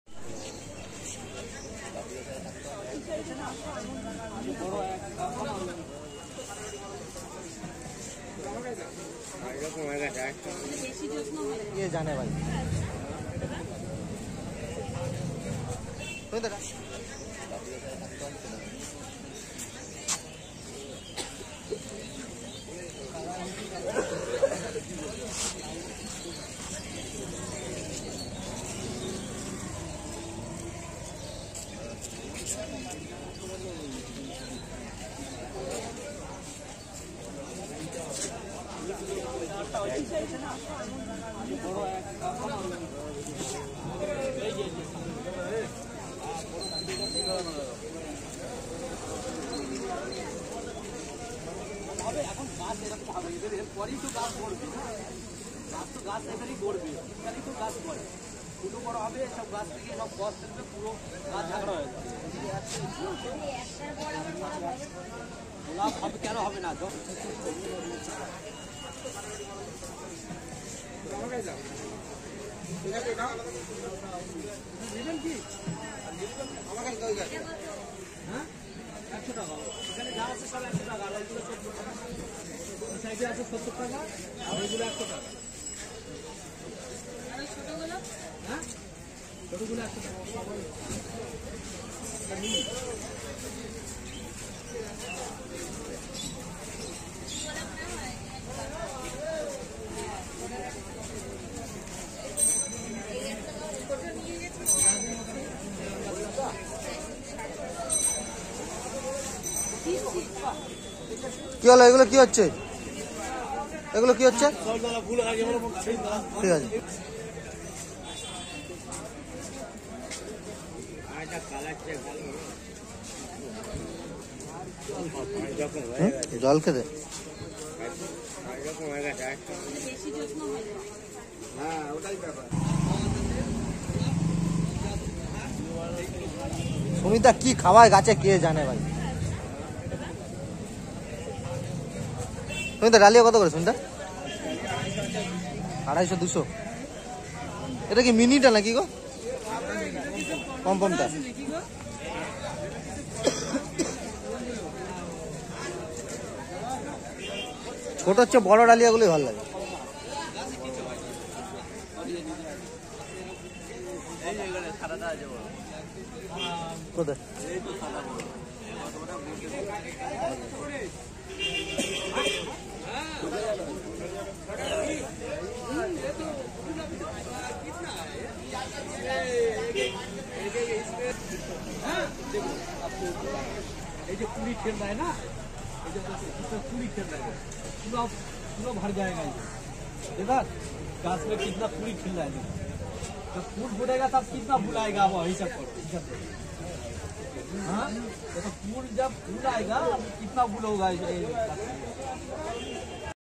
3 December Morning Galiff Street Visit | Winter Season Flower Plants In Tala Haat
3 December Morning Galiff Street Visit | Winter Season Flower Plants In Tala Haat

.🙏 Bangla No.1 🙏.